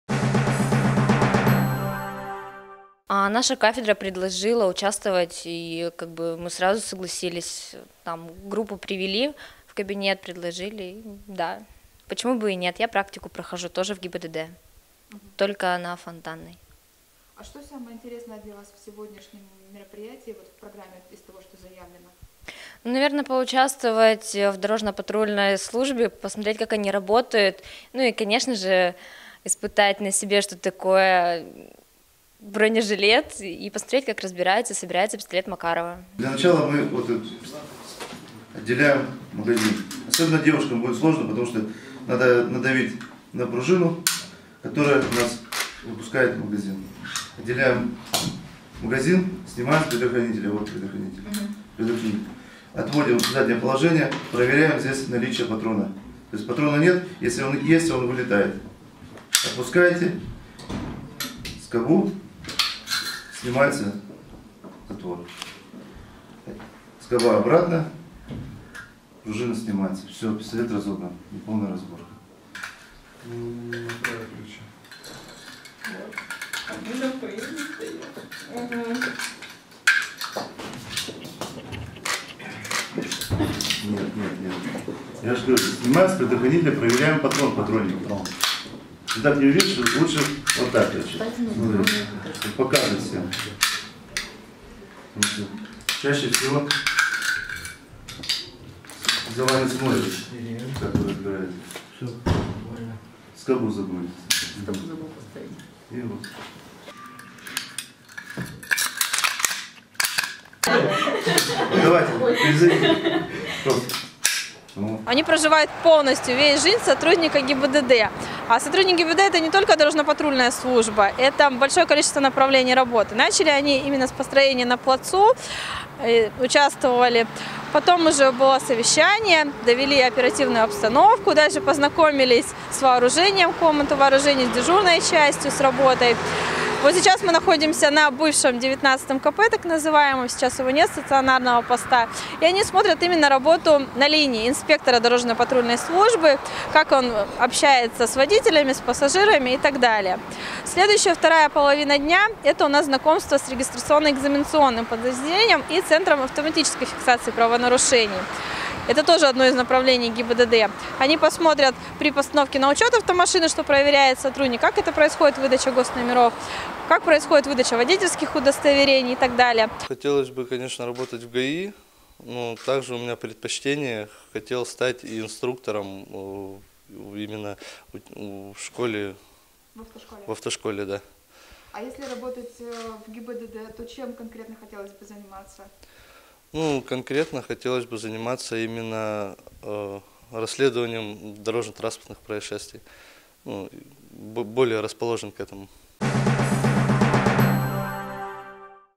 VL ru Владивостокские студенты «прожили» один день как сотрудники ГИБДД
Во вторник, 20 января, во Владивостоке семь учащихся вузов стали участниками «Студенческого десанта». Это всероссийская акция, в рамках которой учащиеся высших и средних специальных

учебных заведений проходят стажировки в разных подразделениях ОВД. Владивостокские студенты «прожили» один день как сотрудники УГИБДД МВД России по Приморскому краю.